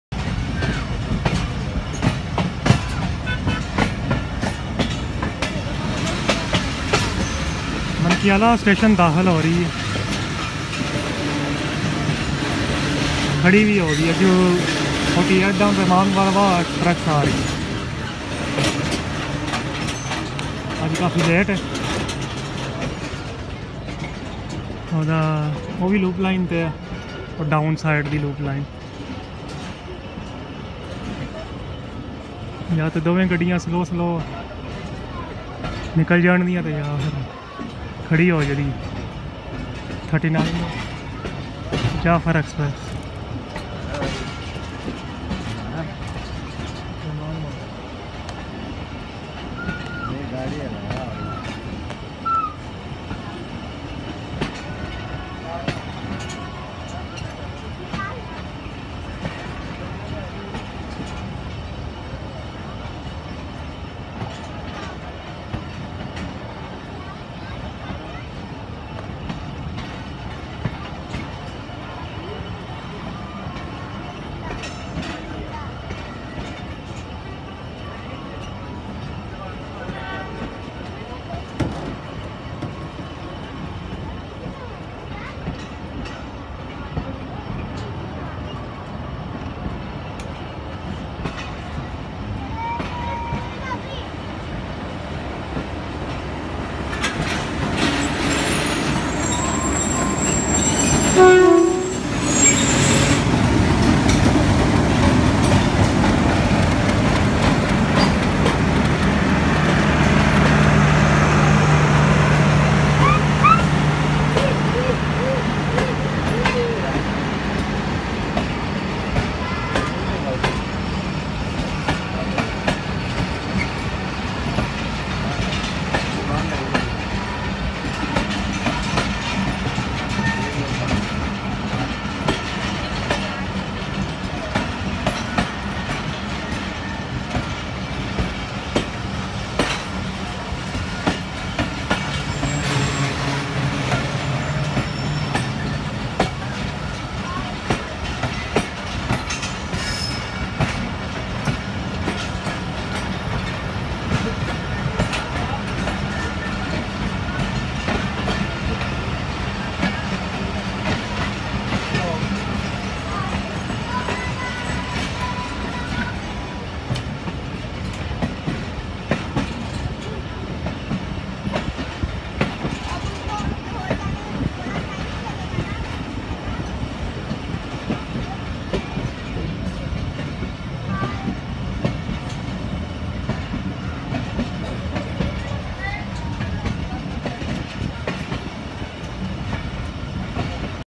BOTH EXPRESS TRAIN || LOOP LINE ACTION || AT MANKIALA RAILWAY STATION
This Video Action Two Express Train 39up Jaffar Express and 48Dn Rehman Baba Express on Loop Line Slow Pass at Mankaila Railway Station

time 1535